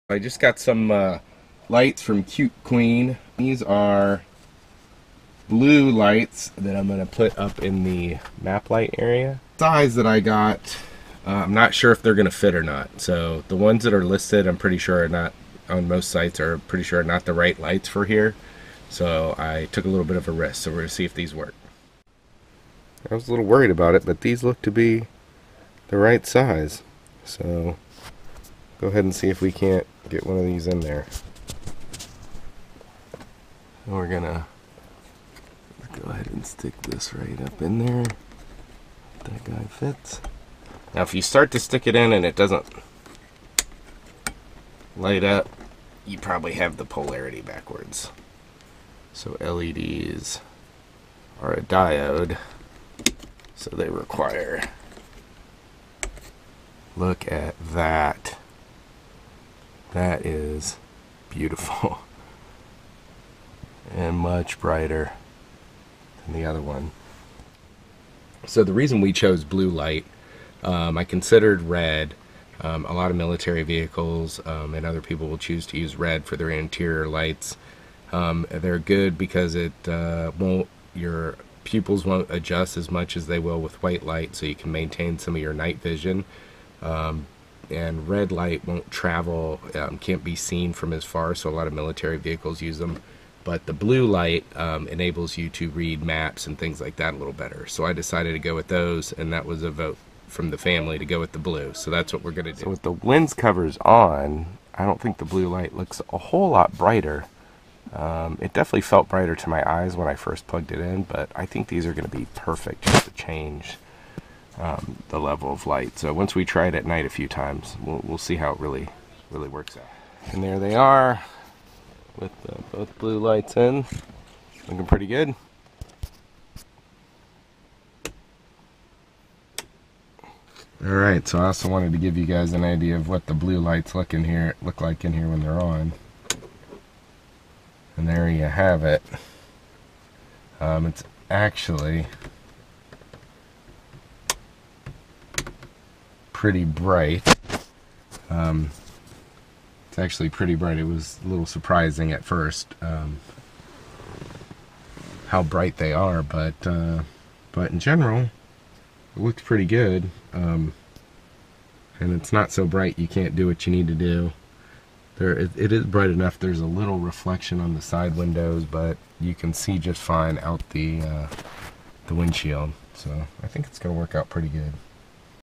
T10 194 Blue LED Map Light Land Cruiser 100 Series Install
I am slowly upgrading the lighting in the land cruiser.  We installed 8x T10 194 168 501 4-smd 3528 LED Car Light Bulb Blue in the in the front map lights.  We considered red but decided on blue after the family voted that way and we knew reading maps at night would be easier with the blue lights.  They are brighter than i expected, maybe a bit too bright but ok.